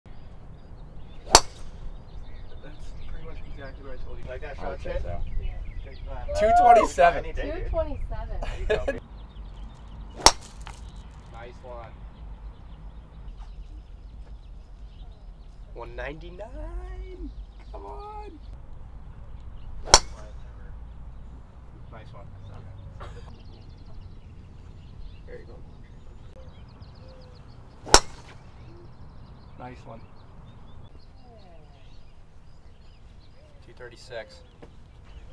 Team TaylorMade Junior Long Drive Competition | TaylorMade Golf
These junior golfers are taking out their Stealth 2 Drivers and participating in a little long drive competition at The Kingdom in Carlsbad. These kids can stripe it and thanks to the most extensive selection of in-line product customizable to junior specs, they're swinging clubs fit for their game.